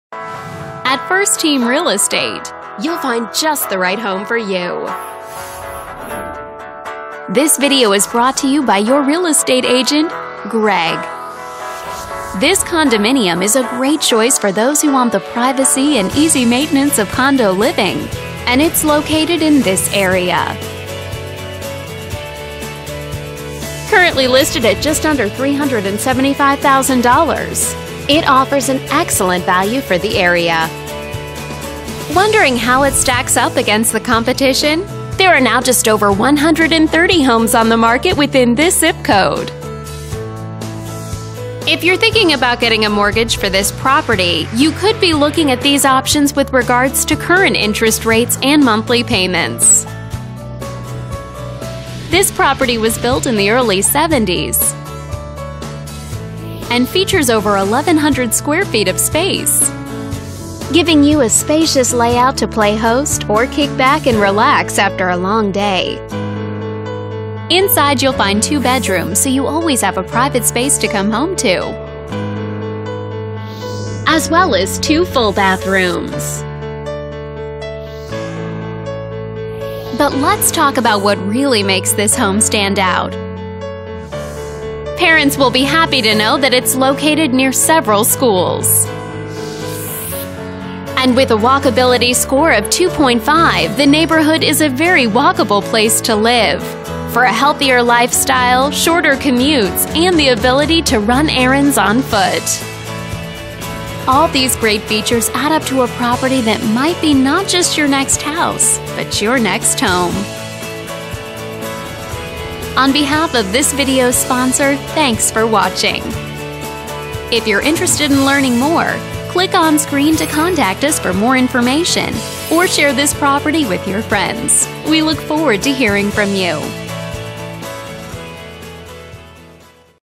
12400 Montecito Road
Welcome to the Rossmoor Chateau Condominiums. This French styled complex has all of the amenities that you would expect. It includes a pool and jacuzzi, BBQ's, clubhouse and weight-room. The condo is located on the top floor with a very nice view. It was recently refurbished and is in very nice condition. The condo has a fireplace, FAH, newer carpets and paint and smooth ceilings. It is located right behind the Rossmoor Shopping Center,  and close to the beach, freeways and Rush Park.  AND...it is located in the highly regarded Los Alamitos School District !!!